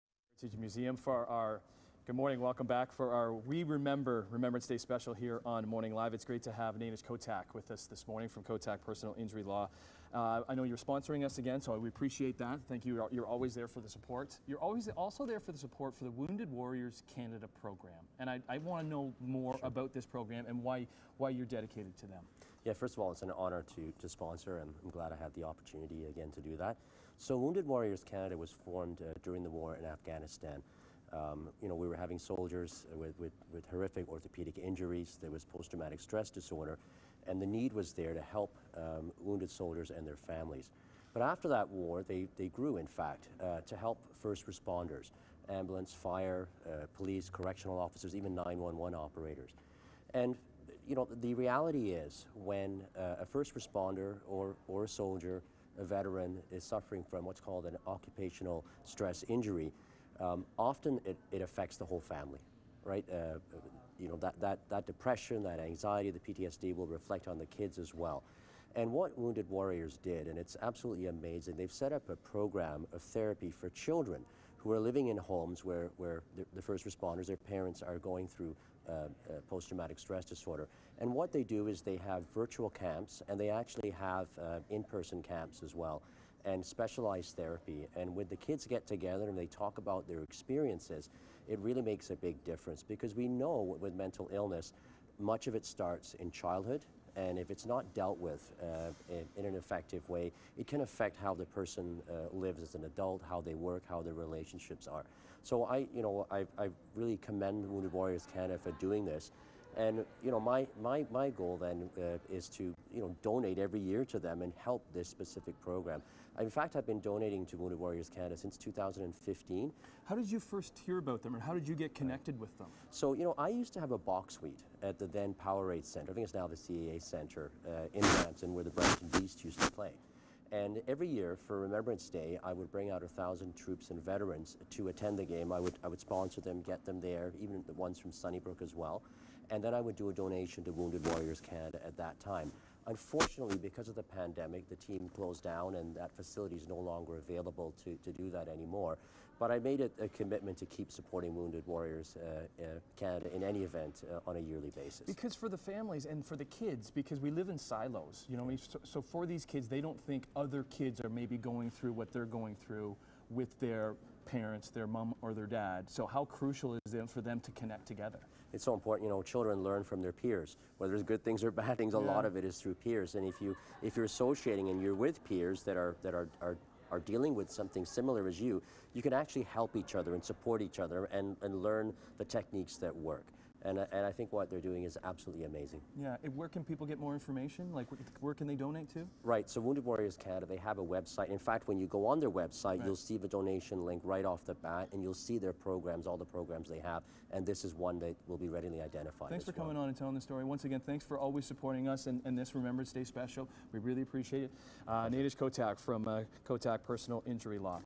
Kotak Law honors Remembrance Day with donation
With today being Remembrance Day Kotak Personal Injury Law made another donation.  They partnered with Wounded Warriors Canada to make a difference for military veterans, first responders, and their families. Nainesh Kotak, a disability and injury lawyer joined us to tell us more.